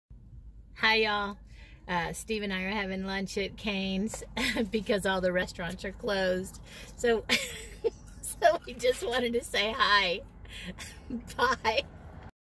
My Steve and I having Cane's lunch
Covid 19 lunch in the vehicle!